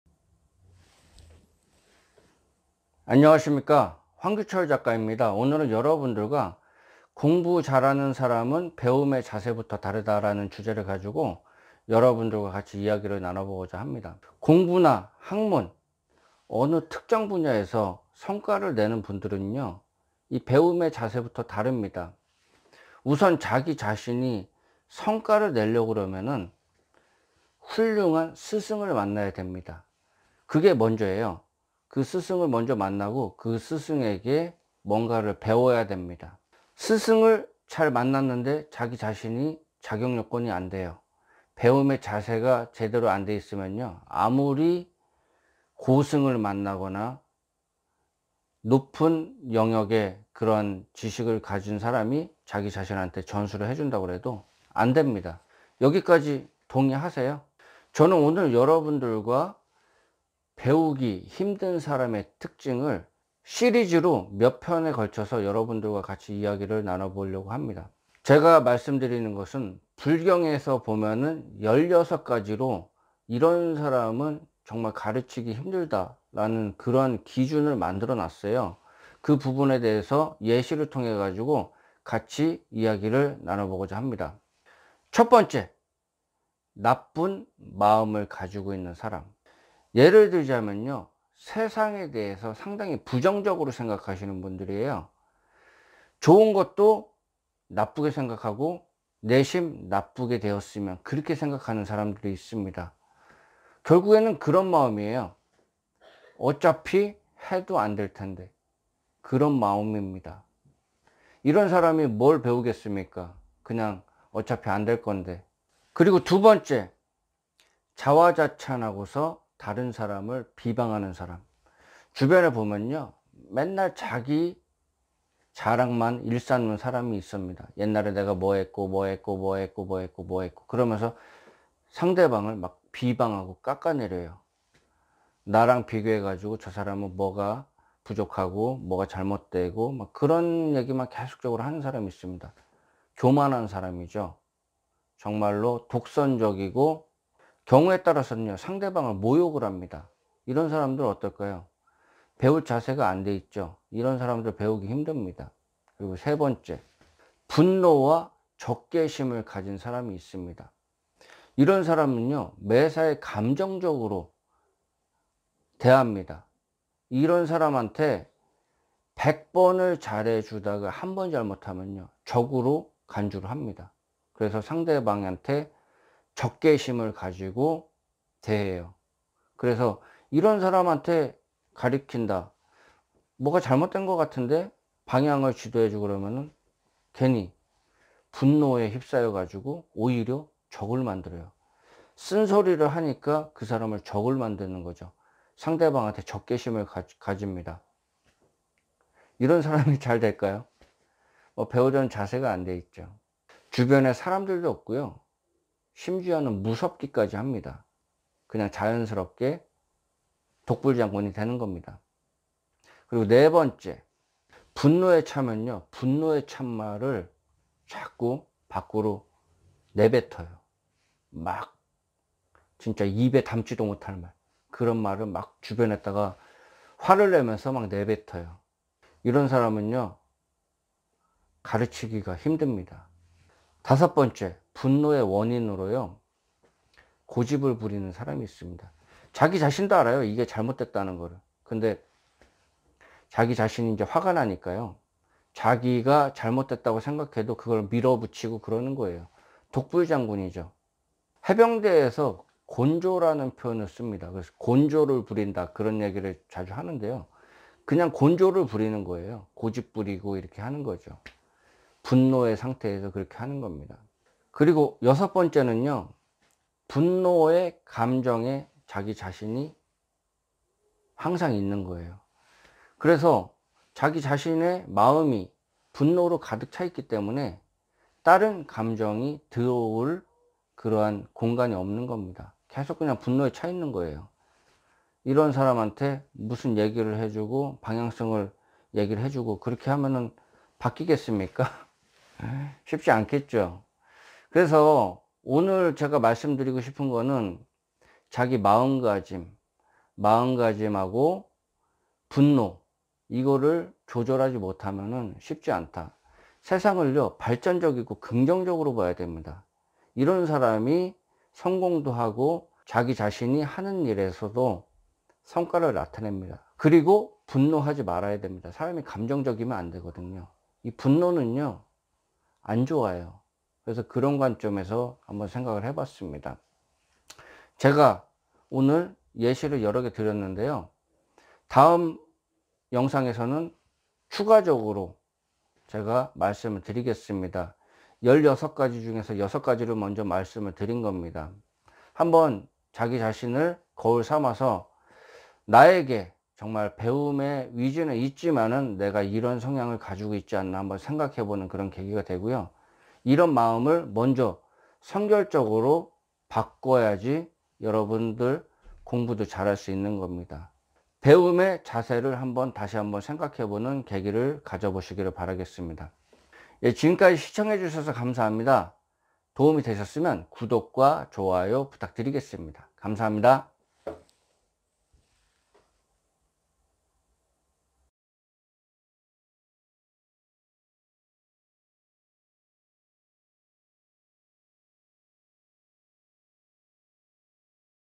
공부 잘하는 사람의 배움의 자세에 대한 첫번째 이야기
공부 잘하는 사람의 배움의 자세에 대한 첫번째 이야기에 대해서 정리해 보았습니다.